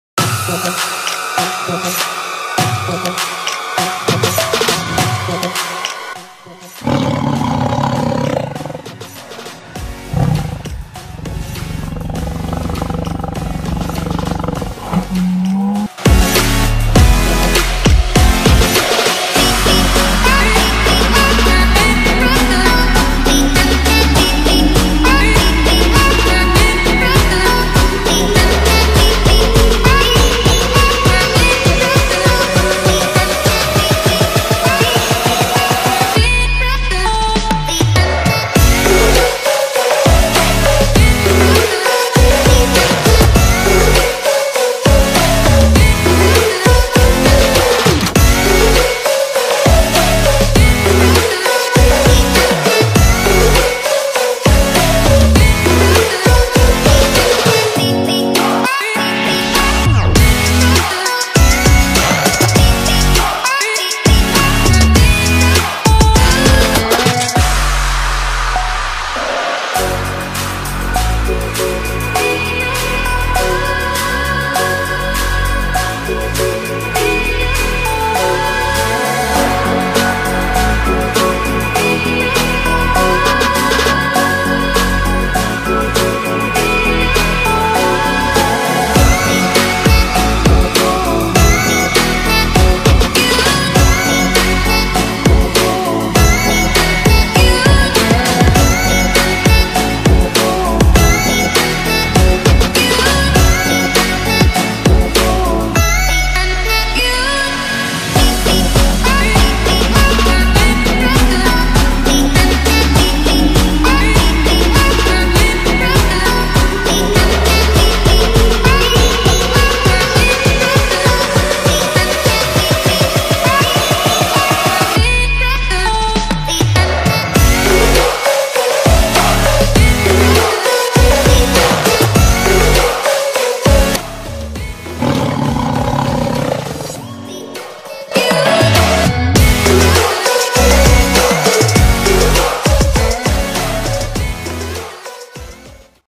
FACEPAINTING MAKEUP INSPIRATION
Ohalo beauty ,, akhirnya aku upload video tutorial ttg facepainting / face art yg selama ini kalian tunggu-tunggu ^_^ kali ini aku shared ke kalian berupa inspirasi imlek & shio harimau hehehe meskipun taun ini shio babi :D:D

semangattttt !!!

Camera : Samsung J7 Pro . .